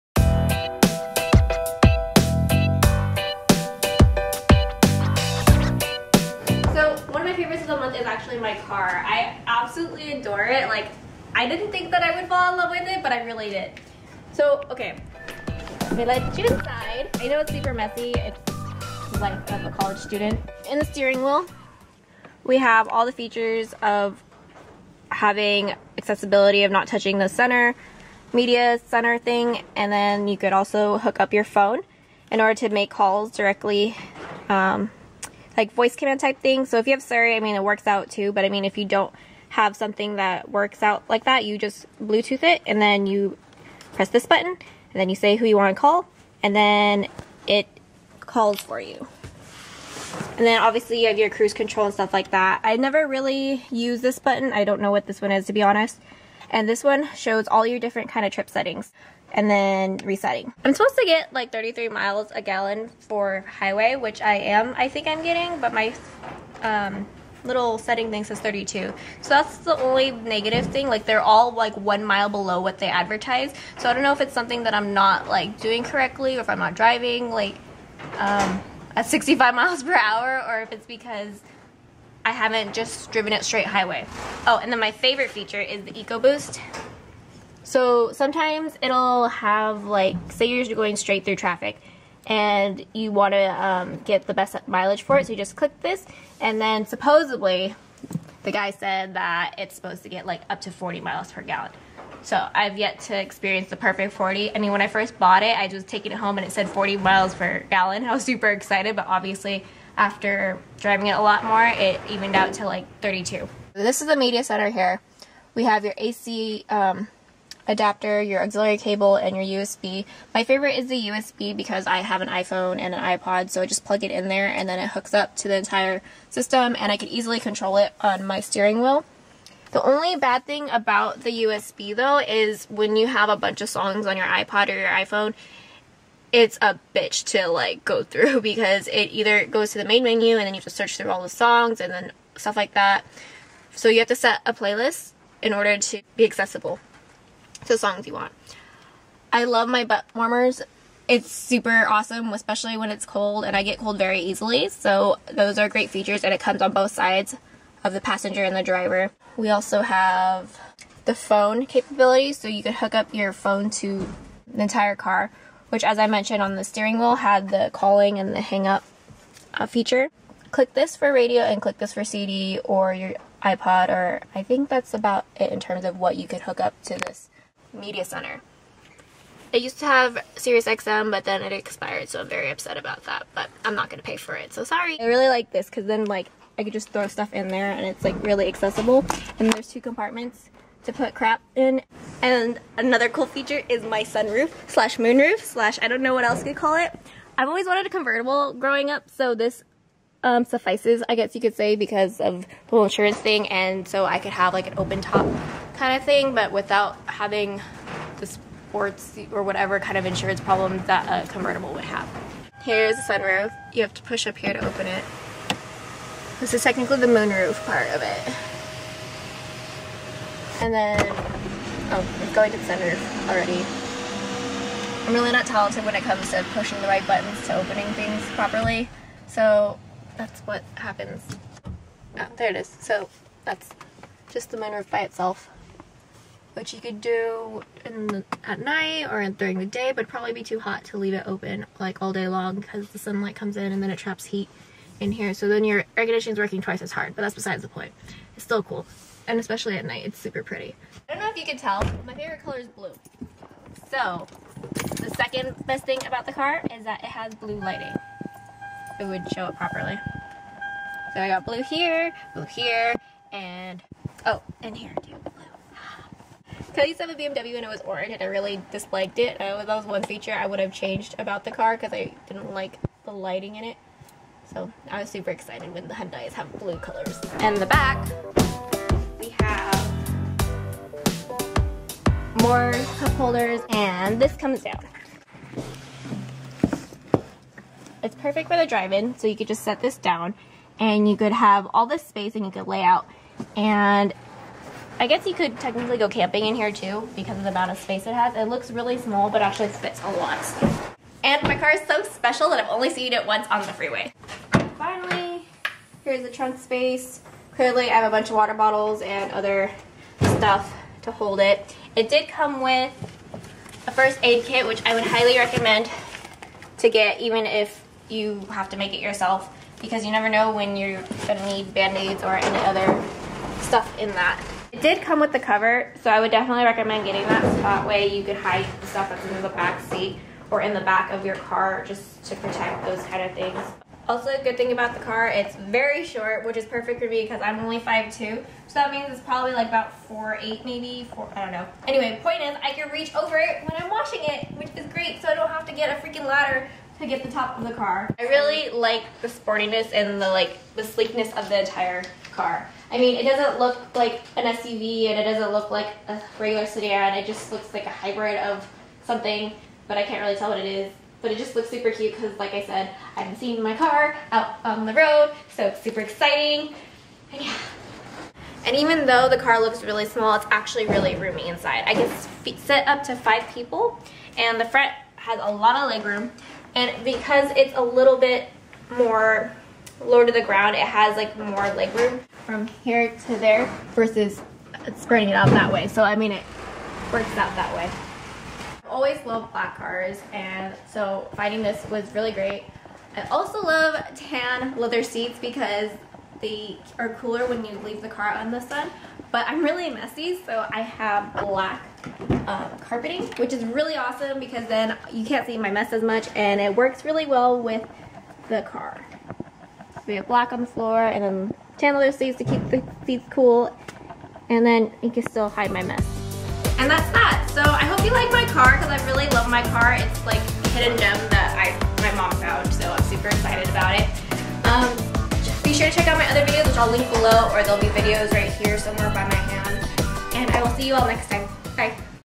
CAR TOUR! | Brittnissx3
I N S T A G R A M // @brittnissx3
T W I T T E R // @brittnissx3

I made this video for those of you who are considering to buy the car and needed a second opinion when making your car purchase, and if you don't want to go to the dealership to deal with the sales associates.

Maker: Hyundai
Model: Elantra GT
Year: 2013